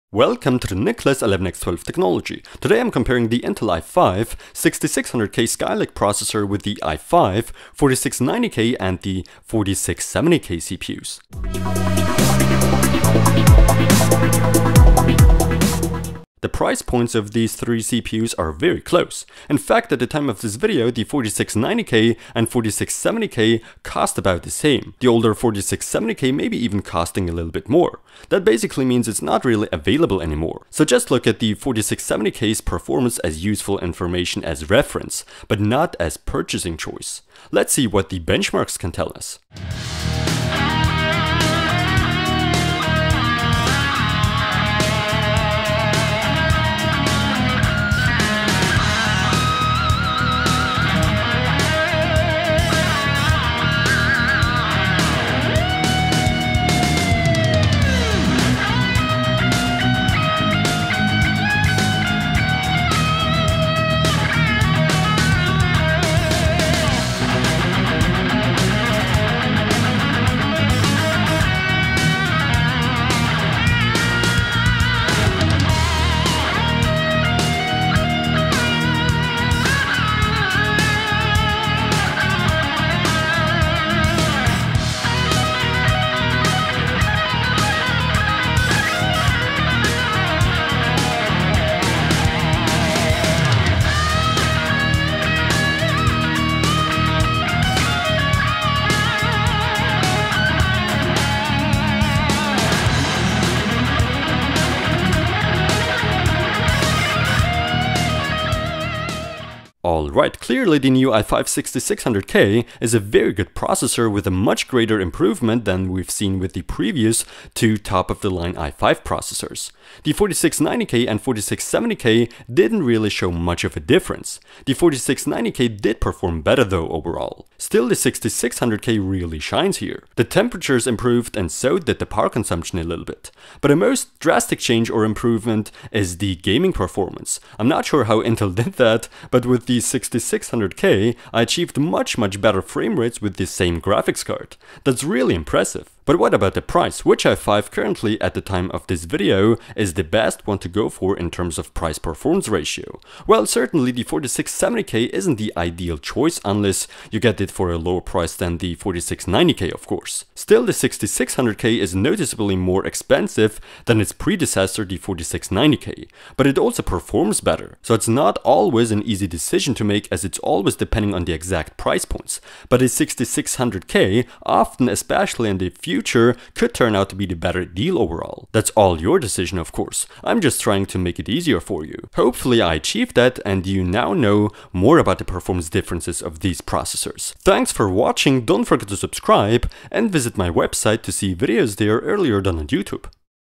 Intel i5-6600K vs i5-4690K vs i5-4670K
Nicolas11x12 comparing the Intel i5-6600K with the i5-4690K and i5-4670K Processors.
How well do these CPUs perform?